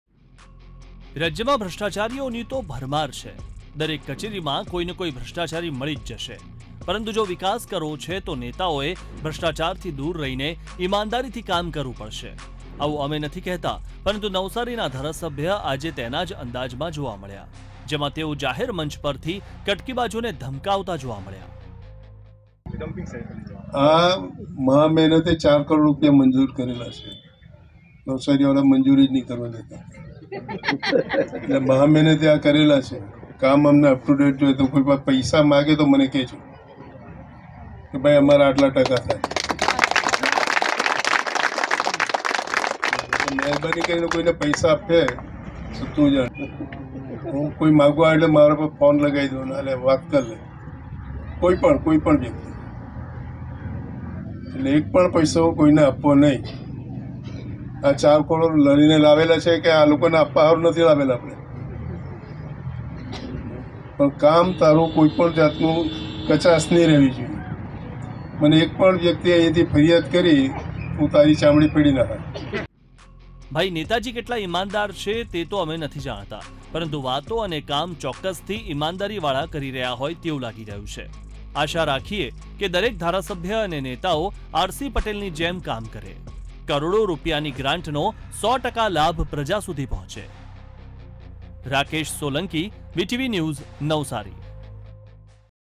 'કામ બાબતે કોઈ પણ ફરિયાદ આવી તો હું તારી ચામડી ફેડી નાખીશ', જલાલપોરના MLA આર. સી. પટેલનો દબંગ અંદાજ
જલાલપોરના ધારાસભ્ય આર.સી.પટેલનો દબંગ અંદાજ જોવા મળ્યો.ધારાસભ્યએ તળાવના કામમાં વેઠ ઉતારતા અધિકારીઓ અને કોન્ટ્રાક્ટરને જાહેરમંચ પરથી ચેતવણી આપી.જાહેરમાં કોન્ટ્રાક્ટર અને અધિકારીને ધારાસભ્યએ ખખડાવ્યા.અધિકારીને બોલાવી તળાવના કામમાં કોઈને પણ ટકાવારી ન આપવા ચેતવણી આપી...ધારાસભ્યએ કહ્યું કે, મહા મહેનતે તળાવ માટે 4 કરોડ રૂપિયા લાવ્યા છીએ.કોઈ ટકાવારી માગે તો મને ફોન કરાવજો.4 કરોડ રૂપિયા આવા લોકોને આપવા નથી લાવ્યા.જો કોઈને રૂપિયા આપ્યા તો હું તારી ચામડી ઉખાડી નાંખીશ.ધારાસભ્યની વાતથી સરકારી કામોમાં ભ્રષ્ટાચાર ચાલતો હોવાની ચર્ચા ઉઠી છે.